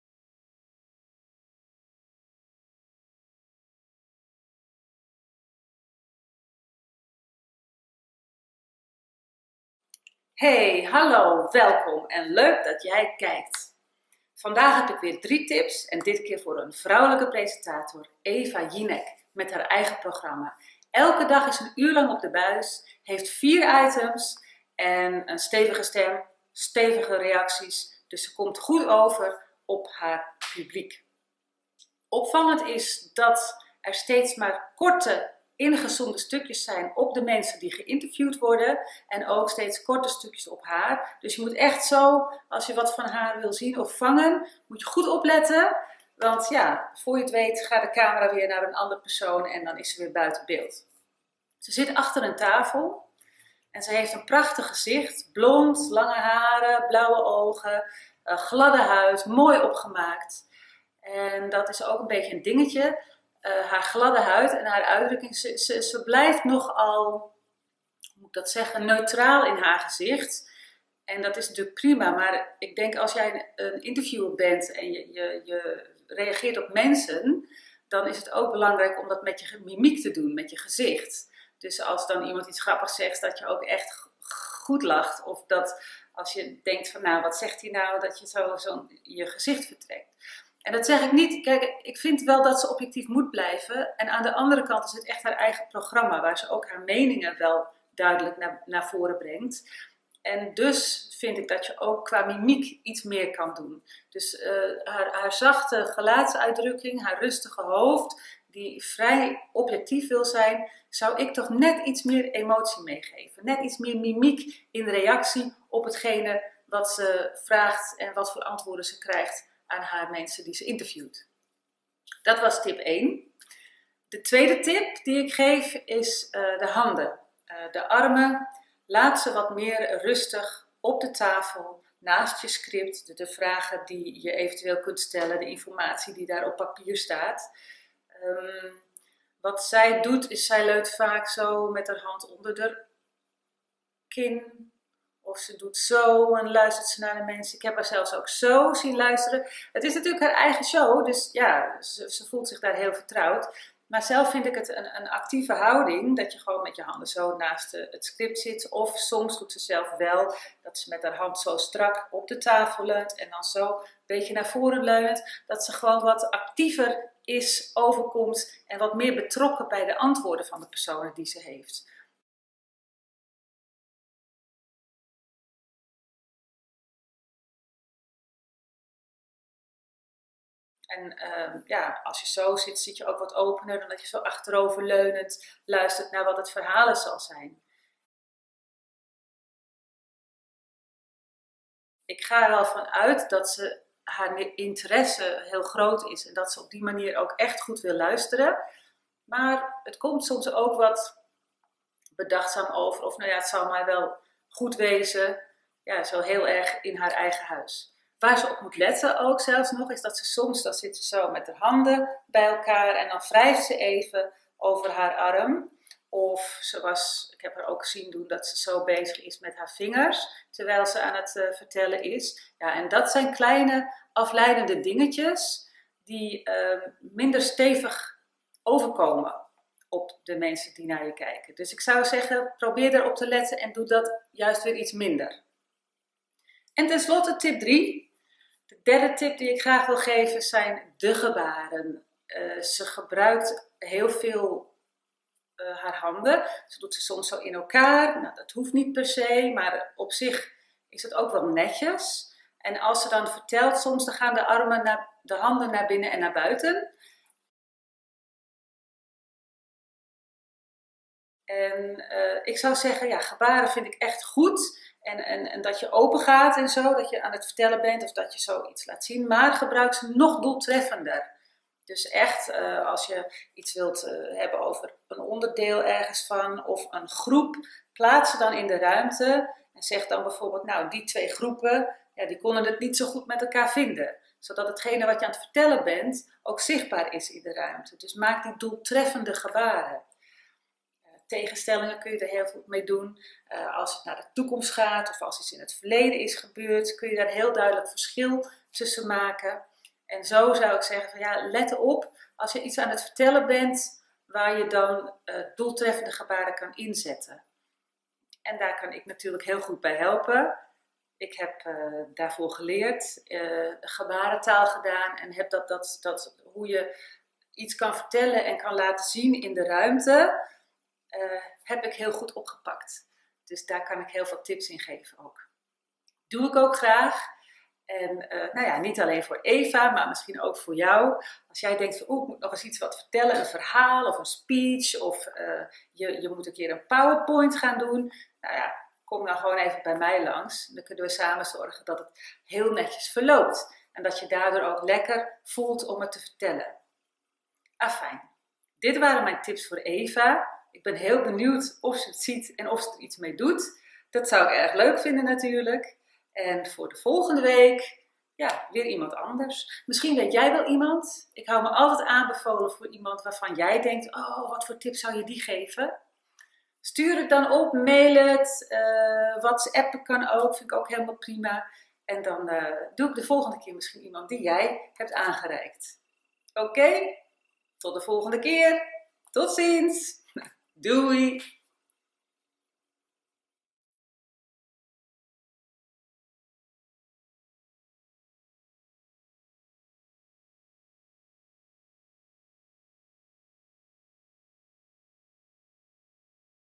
3 presentatietips voor Eva Jinek
Deze week heb ik mijn aandacht gericht op presentator Eva Jinek. De tips die bij mij naar boven komen zijn:
1 - Meer beweging in je gezicht
2 - Handen rustig op tafel
3 - Doelgerichte gebaren
Verder heb in nog wat extra tips geschreven in mijn blog, die vindt je via mijn